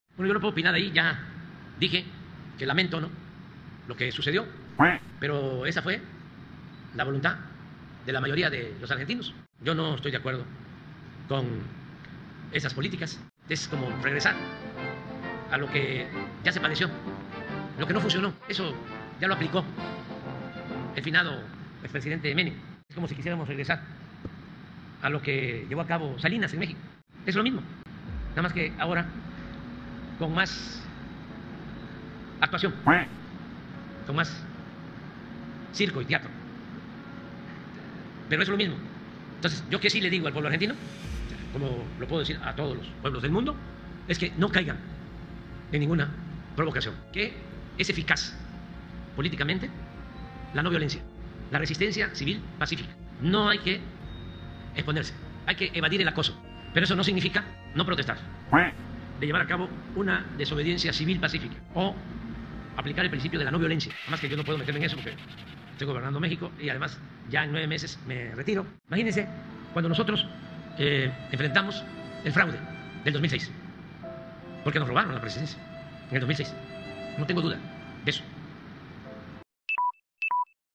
AMLO® recomienda al pueblo de Argentina hacer resistencia civil pacífica contra políticas de Milei
Las políticas de Milei es como regresar al período de Menem, pero ahora con más actuación, circo y teatro. “Les digo al pueblo de Argentina que no caigan en ninguna provocación” 🦆, y les recomienda la resistencia civil pacífica